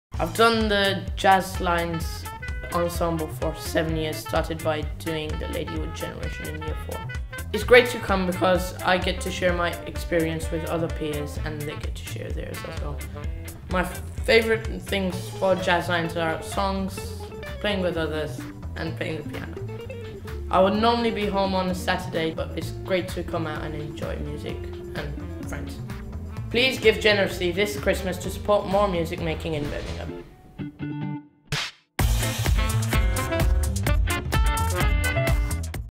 THSH: Meet Nauraiz - The Big Give Christmas Challenge 2019
Meet Nauraiz, one of the participants of Town Hall Symphony Hall's Jazzlines Vocal Ensemble.

This Christmas, Town Hall Symphony Hall are taking part in The Big Give’s Christmas Challenge to double your donations and support the future of music making in Birmingham.

This year, over 100 people joined Town Hall Symphony Hall's free Reggae Choir and sung together for the first time at at Town Hall, whilst over 400 singers from local community choirs took to the stage at Symphony Hall as part of the annual Community Spirit showcase. But there’s so much more we could do…

The redevelopment of Symphony Hall's foyers will create dedicated spaces for learning and participation - that means, we'll be able to introduce over 24,000 young people and adults to a world of music every year.